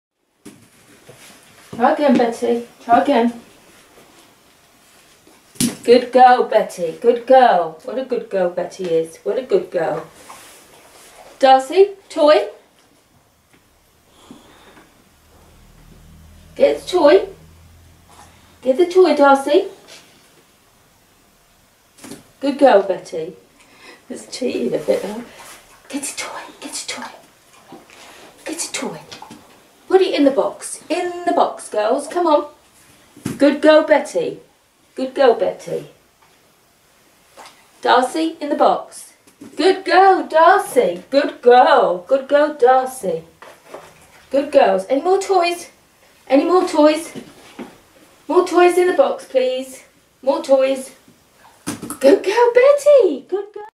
Betty and Darcey putting toys away
Betty cheats !